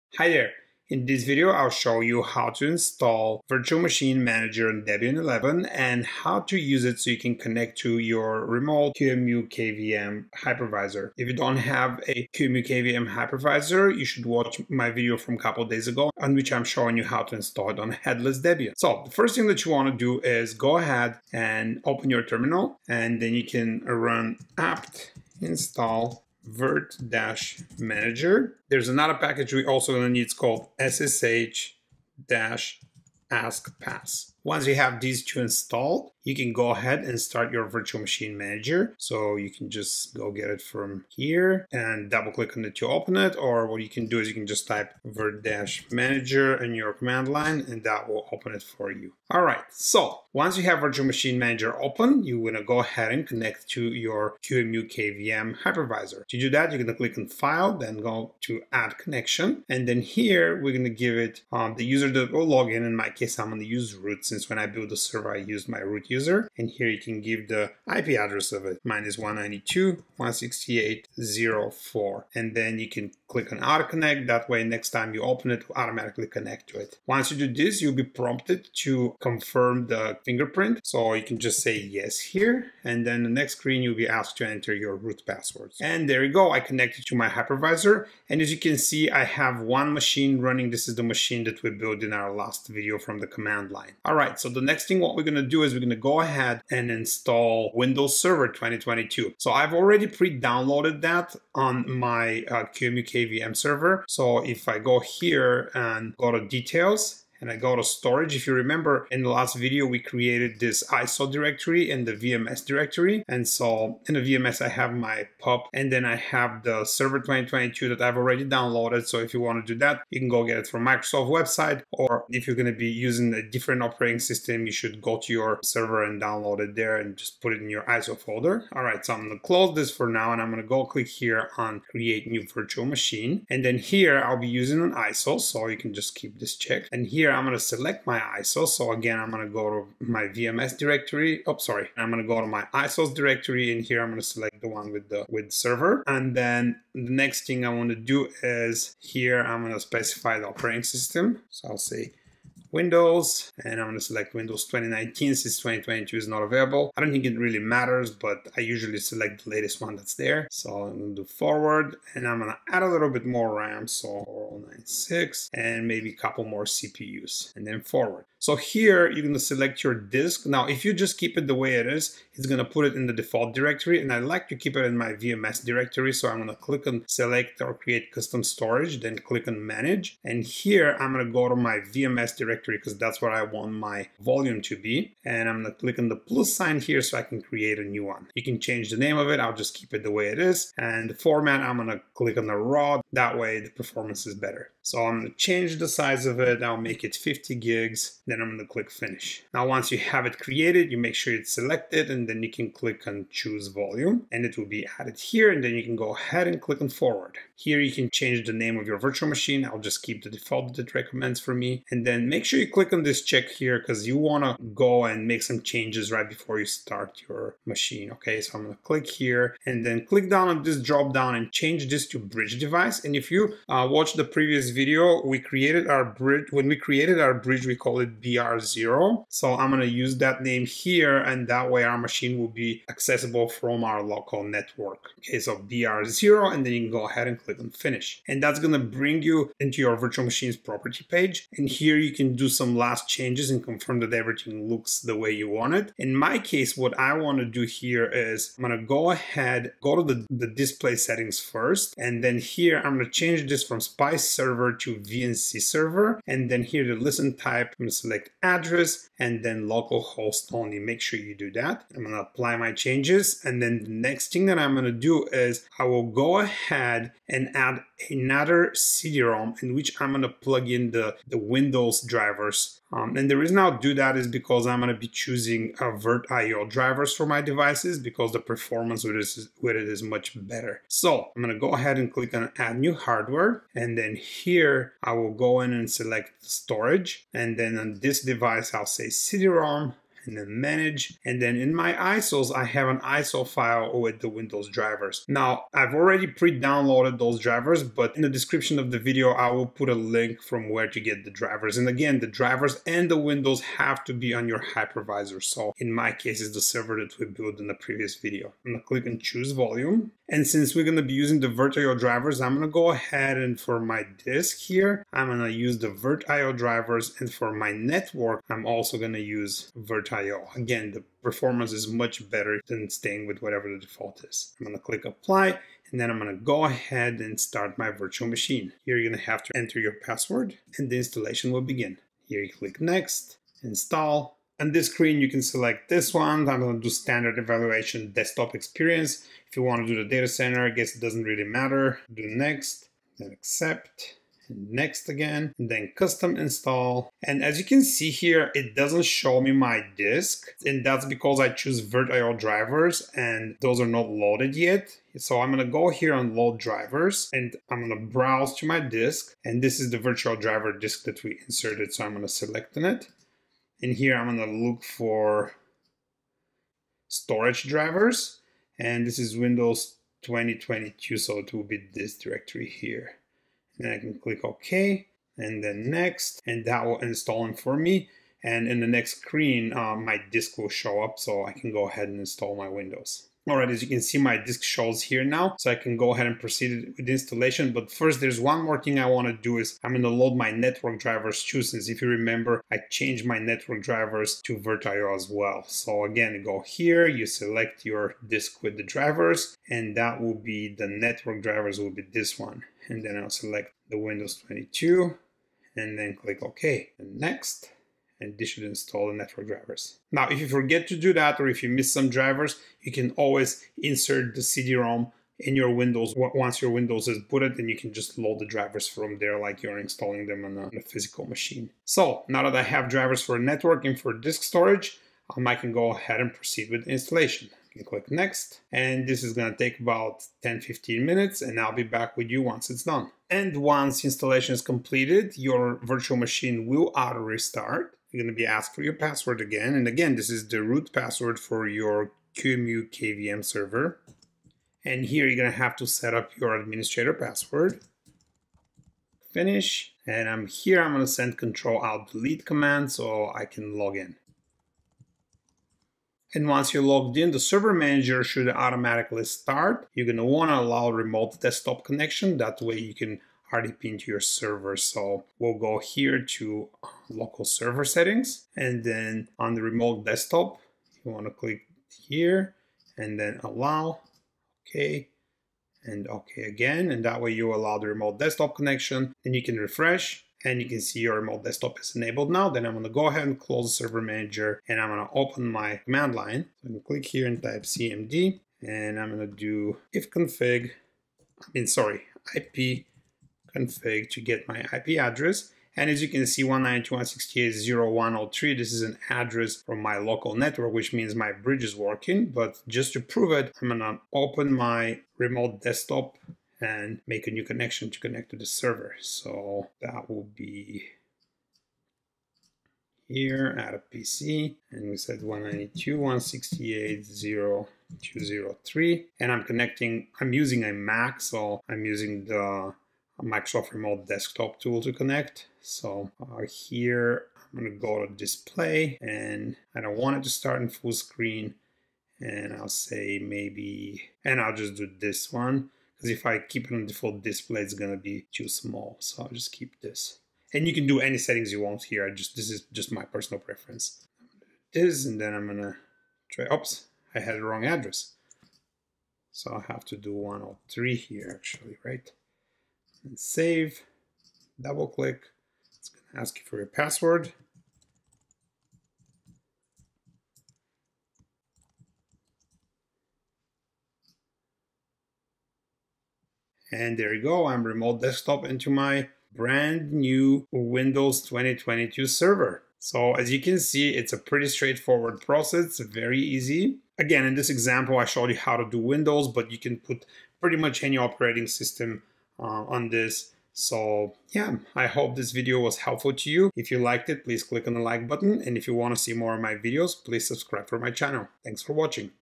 How to create Windows Server 2022 Virtual Server on Virtual Machine Manager on Debian 11.
This video is continuation of where I showed you how to install and configure QEMU / KVM.

In this video I will show you how to instal and configure Virtual Machine Manager, a GUI tool that manages QMEU / KVM virtual machines
How to connect to QEMU / KVM,  create a  Virtual Machine with Windows Server 2022, and install storage and network drivers.